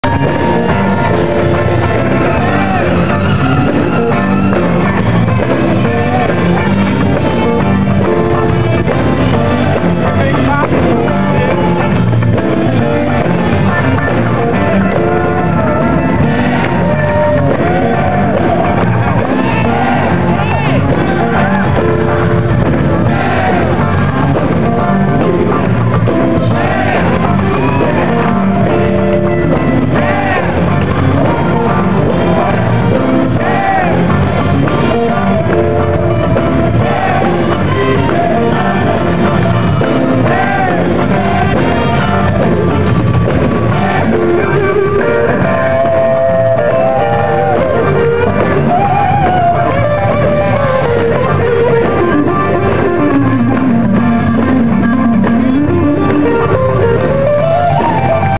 James Brown at Massey Hall Toronto 2004
James comes back for an encore and a bum mic as a Niles Crane wails.